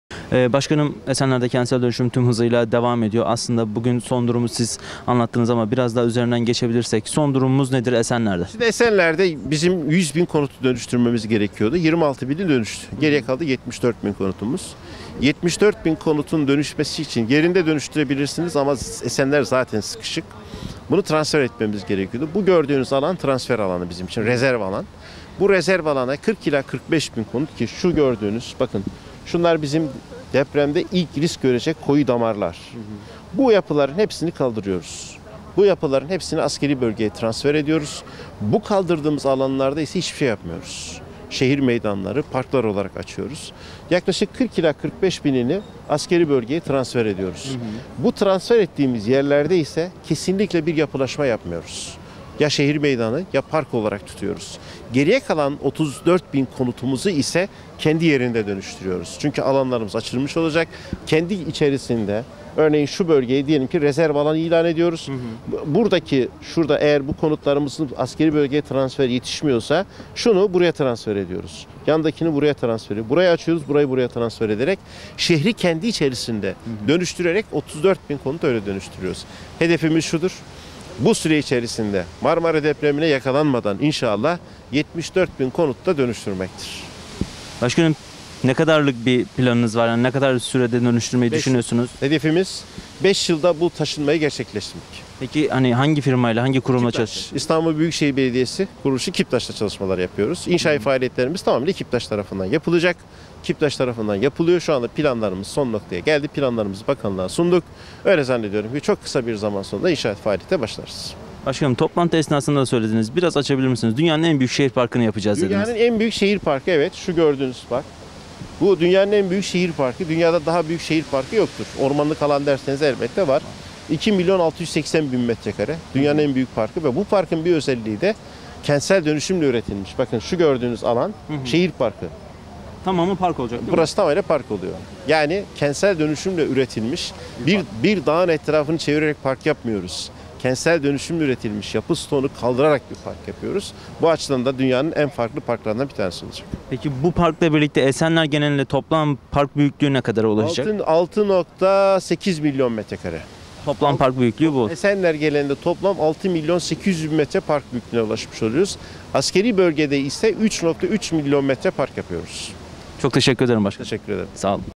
Tevfik Göksu, Esenler'deki dönüşümü ESD'ye anlattı!
Esenler Belediye Başkanı Mehmet Tevfik Göksu, askeri arazide gerçekleştirilecek kentsel dönüşüm projesinin detaylarını emlaktasondakika.com Haber Müdür Yardımcısı Uğurcan Tokay'a anlattı.